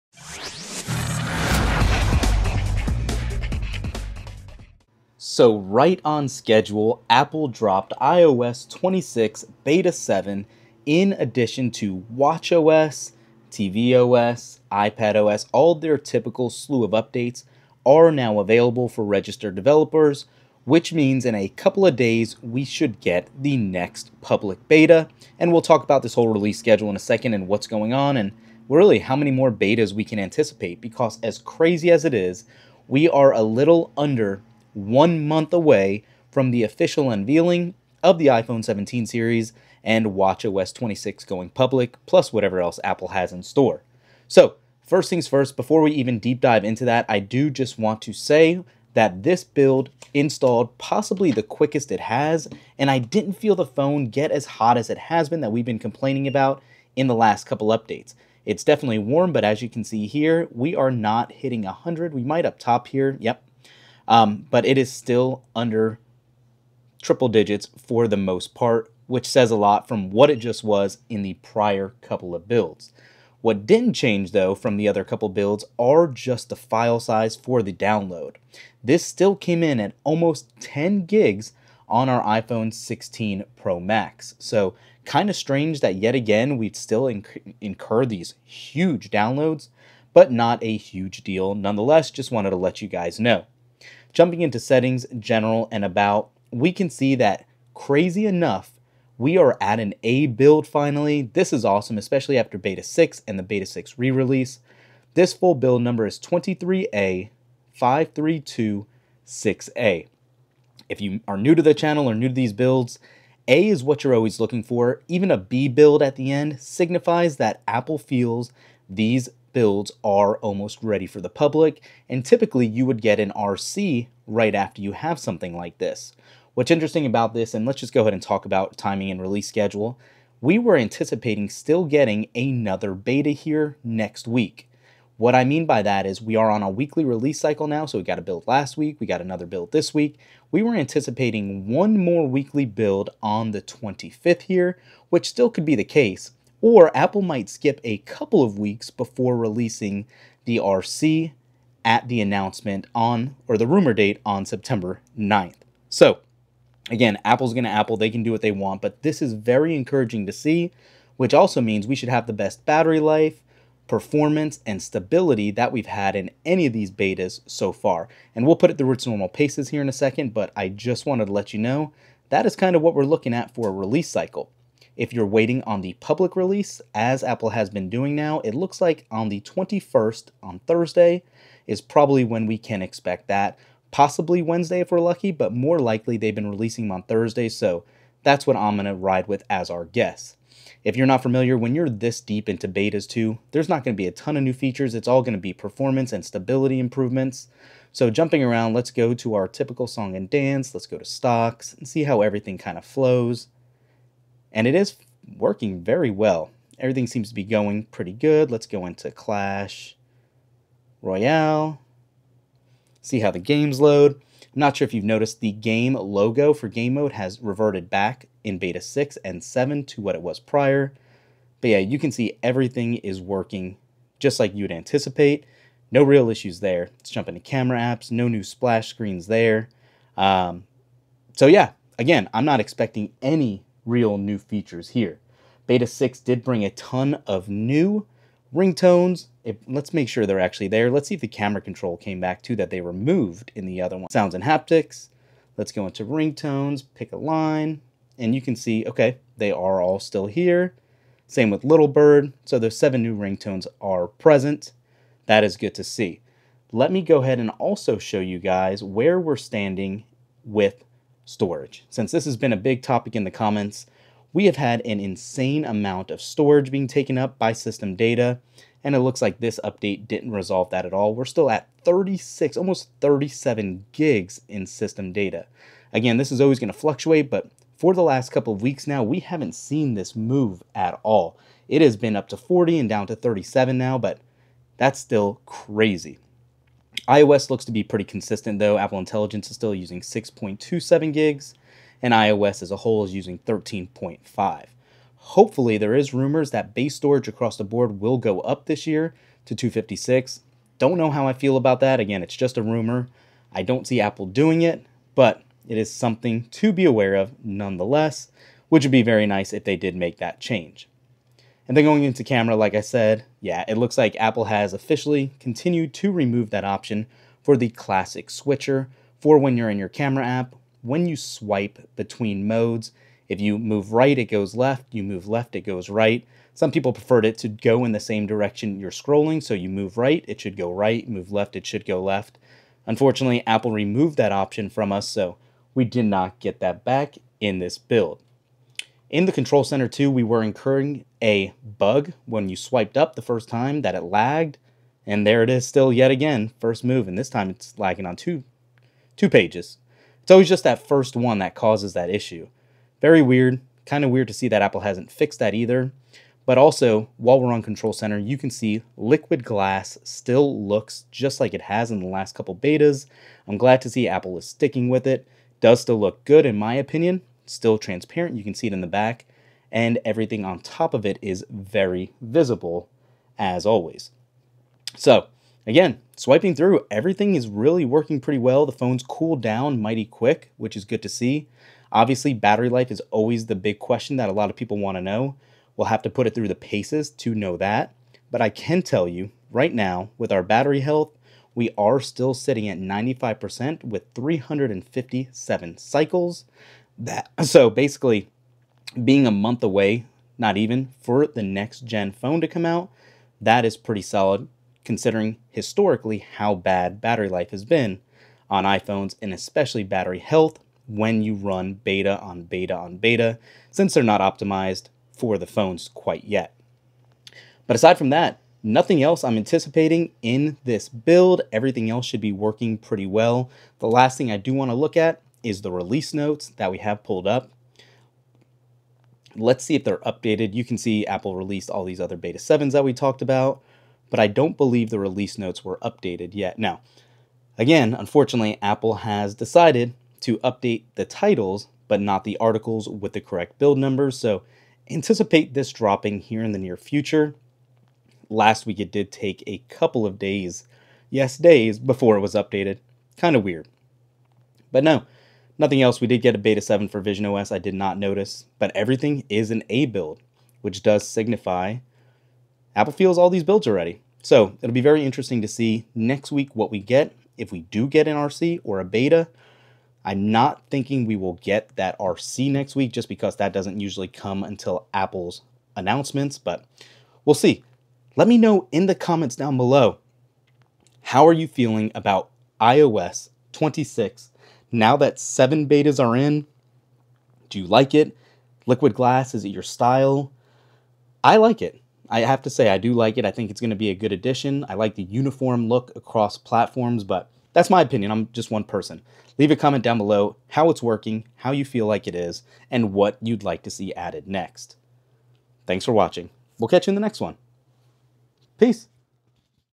iOS 26 Beta 7 Is OUT- Is This The Final Beta!
In somewhat of a surprise, Apple dropped iOS 26 Beta 7 that carries an "a" build at the end signaling that Apple might feel this is ready for the general public. Lets dive in!

65W UGreen Bonus not in video